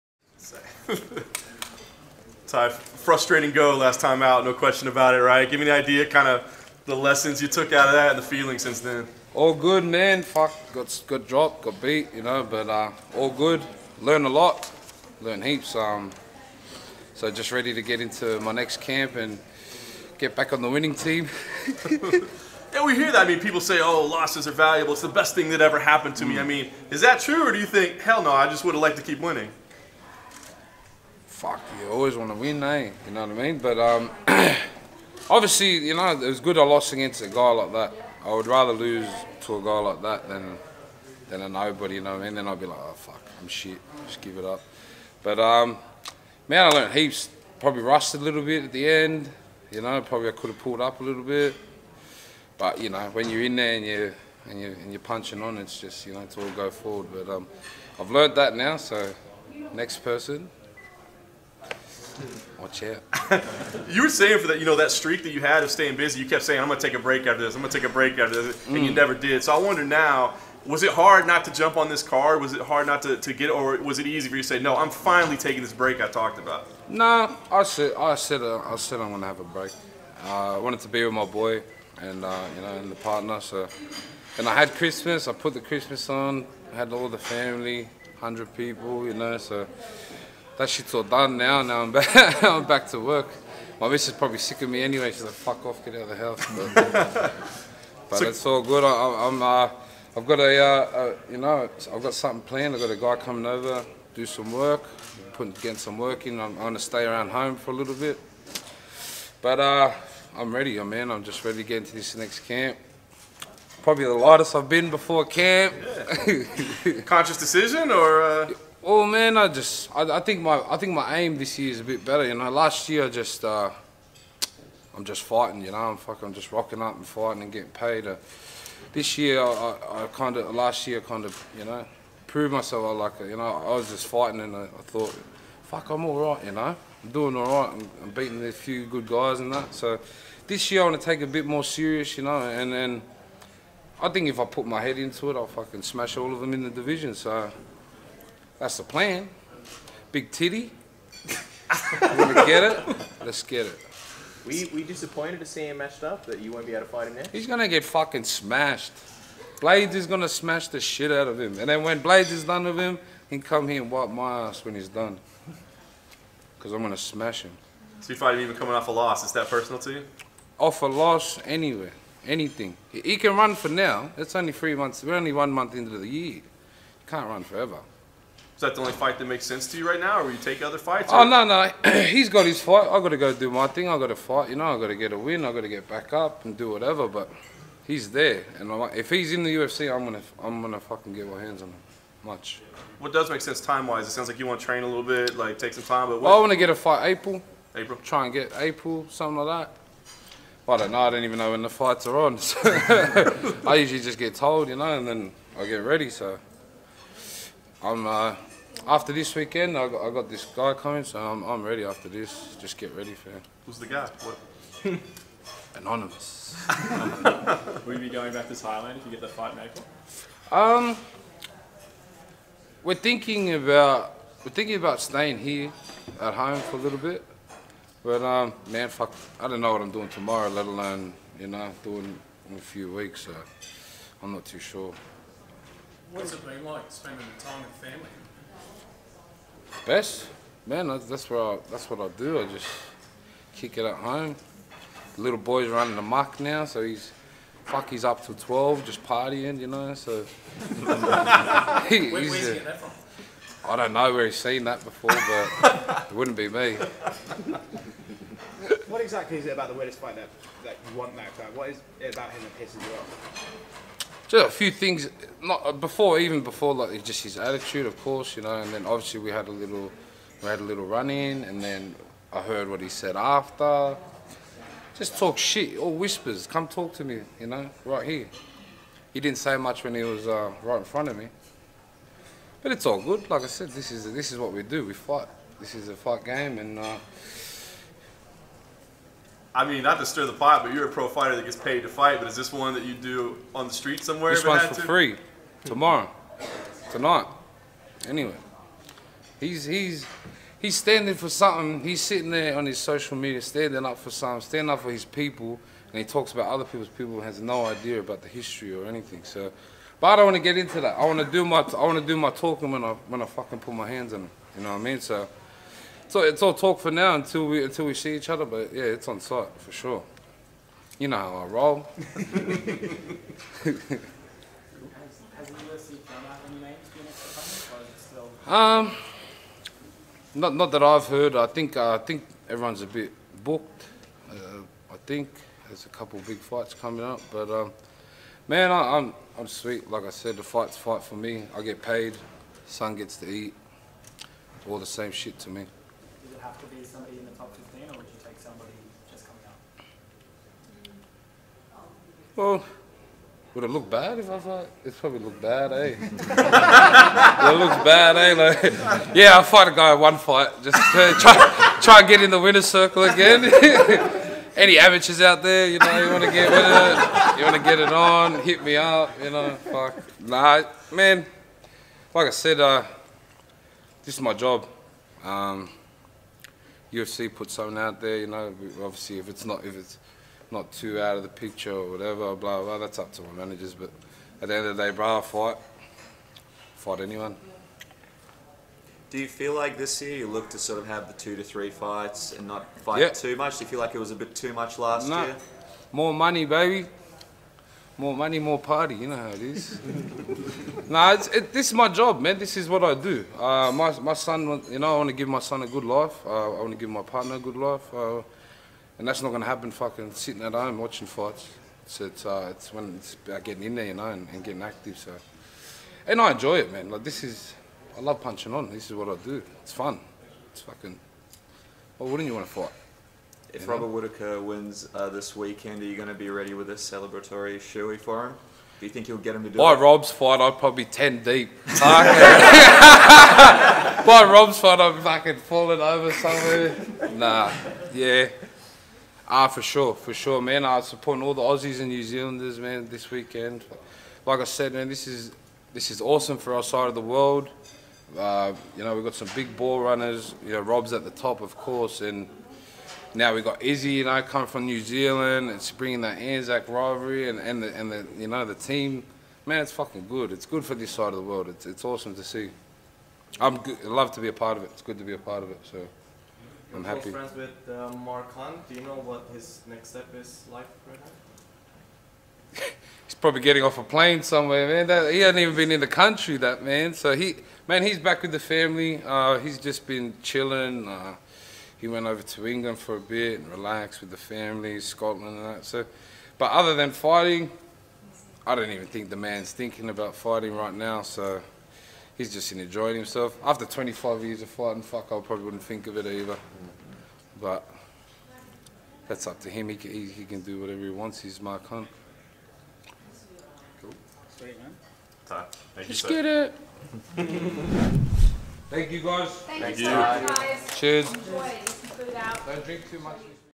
Tai Tuivasa Is Just Too Hot
Tai Tuivasa roasts at the UFC 234 media day in Melbourne.